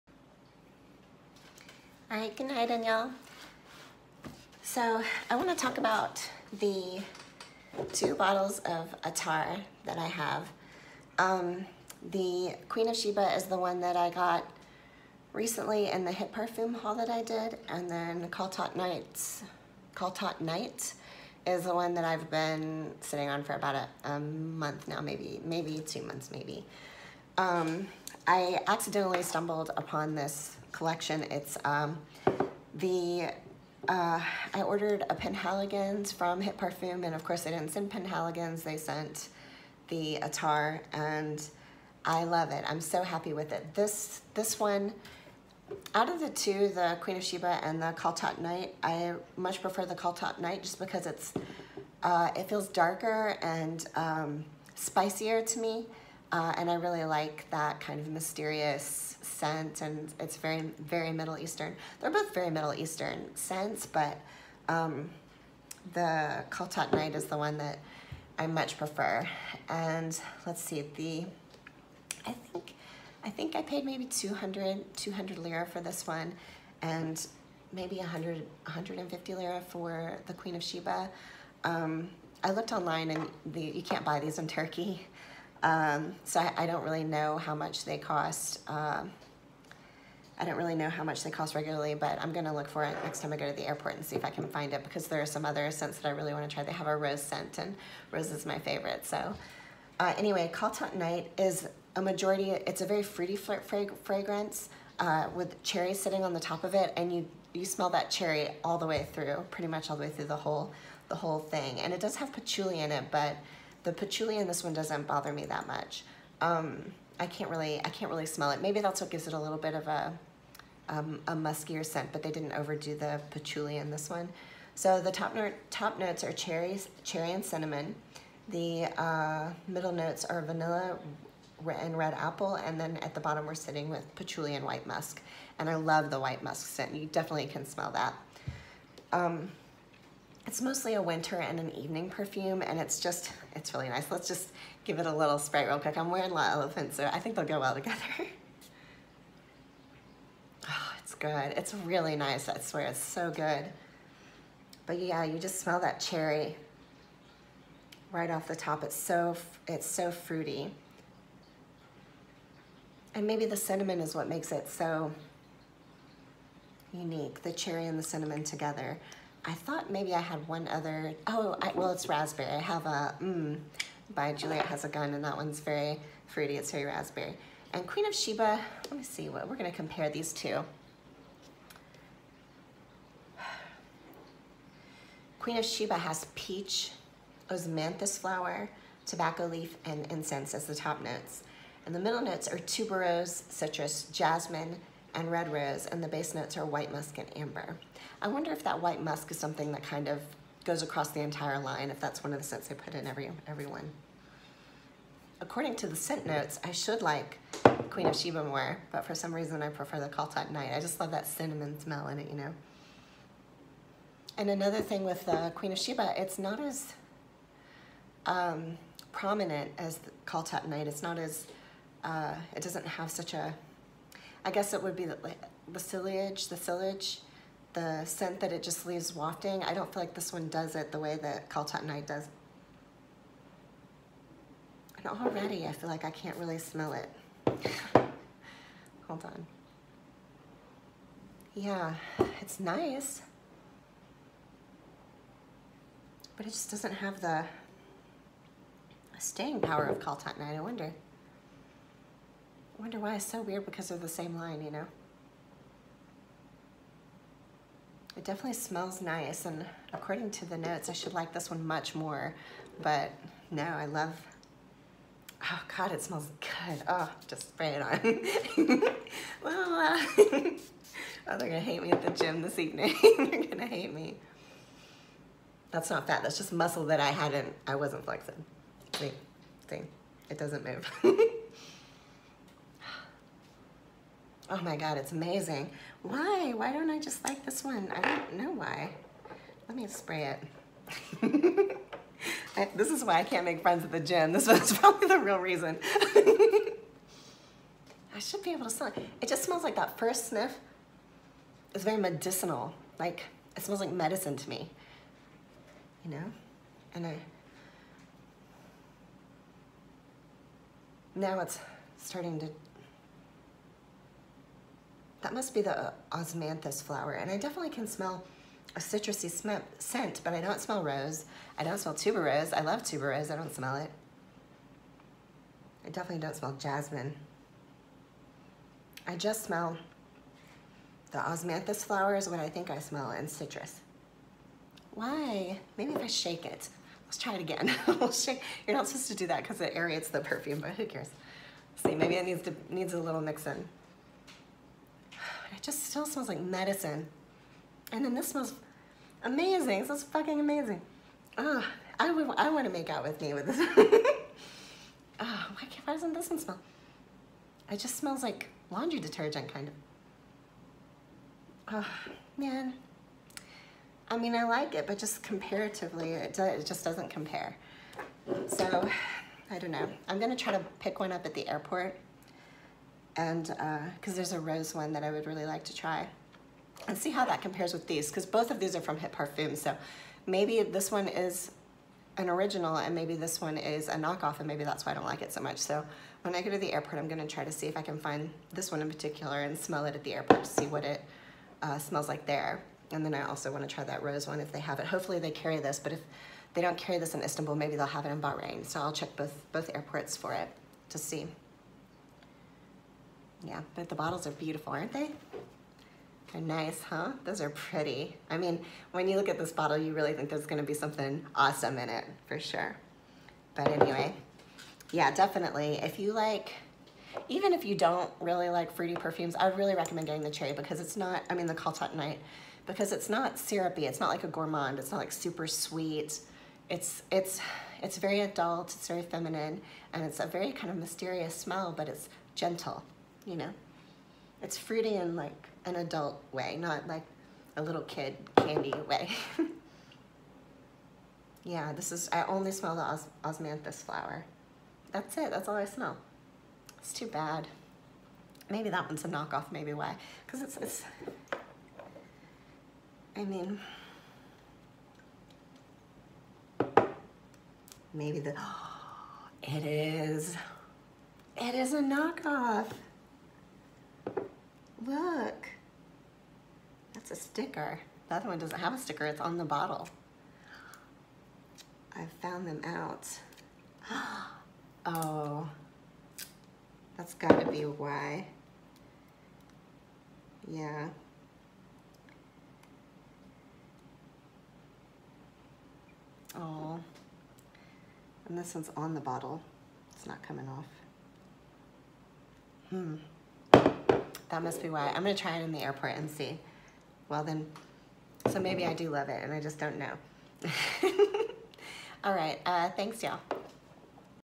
Attar Khaltat Night and Queen of Sheba Perfume Review
Another review where I have no idea what I’m talking about but just happy to be talking about perfume.